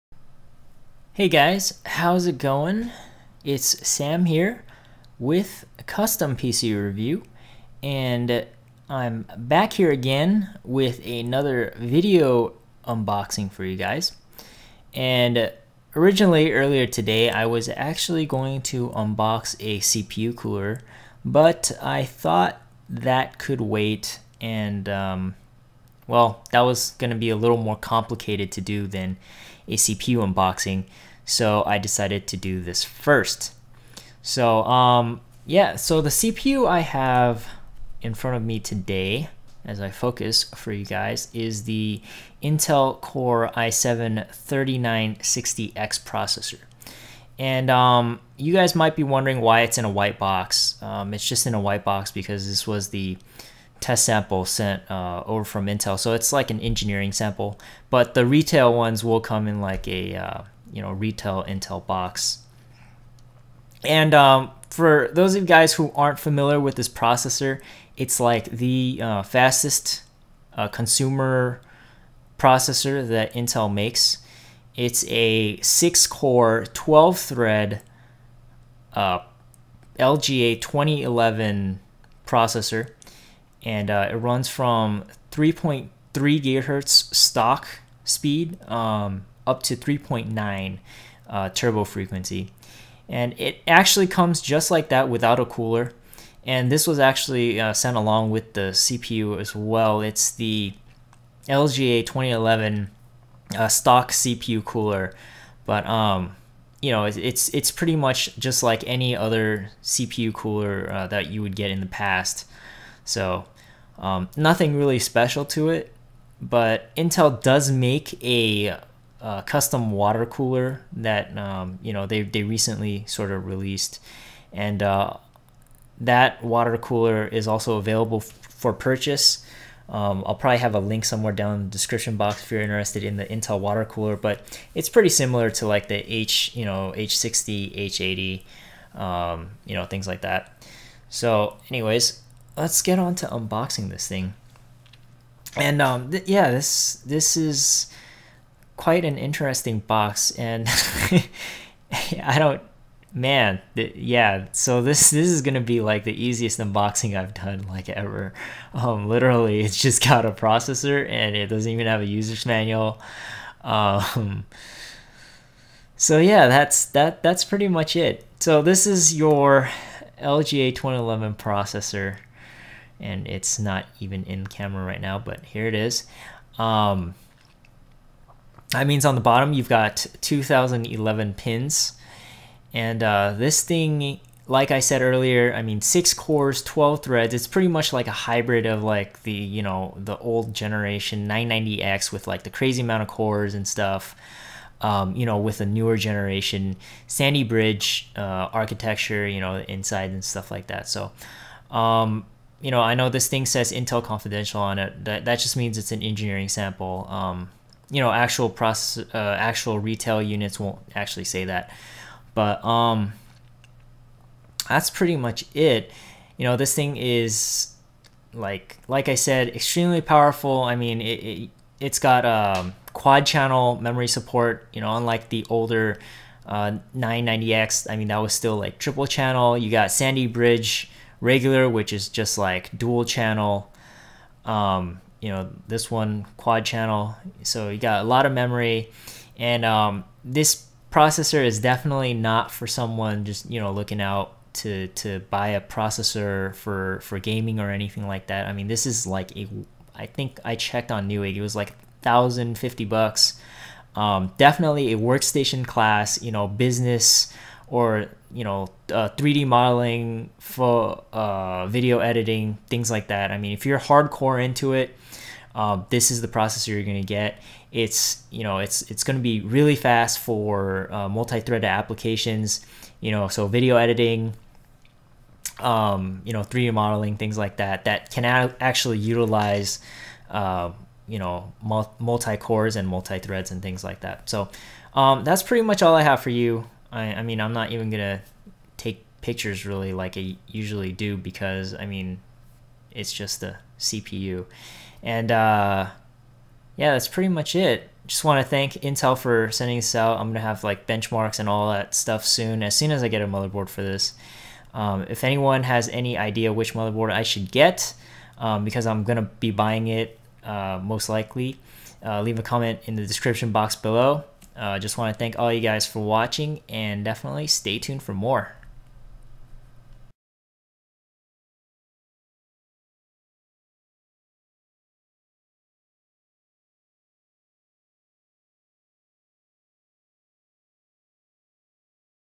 Intel Core i7 3960X Extreme Edition Processor Unboxing + Written Review
A relatively quick unboxing of Intel's flagship Core i7 3960X Extreme Edition Processor. This LGA 2011, 6 core, 12 thread (HT) processor is currently the fastest available consumer level processor available on the market.